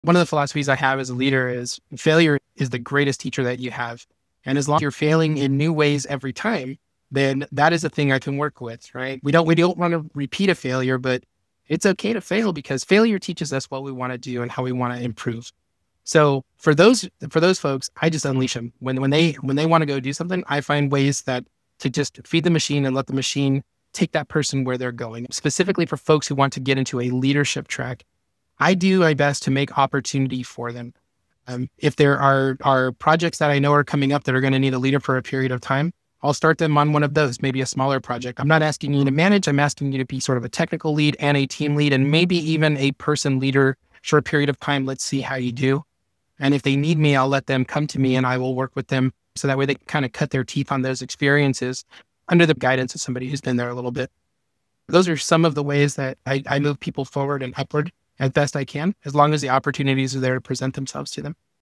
Fail fast, promote faster: How I spot future engineering leaders
Robert Gonzalez, VP of Engineering at SugarCRM, shares how he builds a culture where failure fuels growth and how he identifies future leaders by watching how they handle the unknown.

If you're trying to grow high-performing teams in today’s AI-fueled environment, this is the mindset shift that separates good from great.

🎧 Check out the full Tech Teams Today episode for more insights on global team scaling, AI integration, and human-centered leadership.